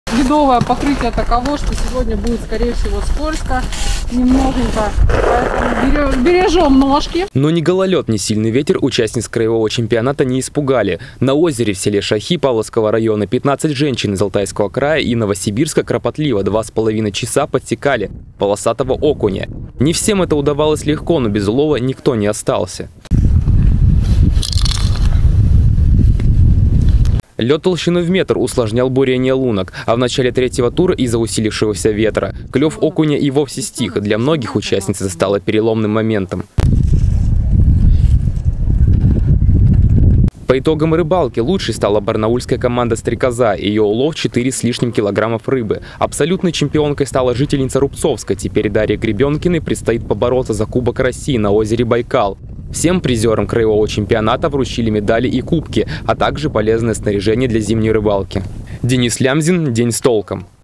Женщины Алтайского края отличились на чемпионате по рыбной ловле
Всего за пару часов 14 килограммов окуня наловили на мормышку жительницы региона на одном из алтайских озер. Совокупный улов не уступает средним результатам мужских турниров по зимней рыбалке. Несмотря на пронизывающий ветер, все участницы показали завидную выдержку, а некоторые даже получили спортивные разряды.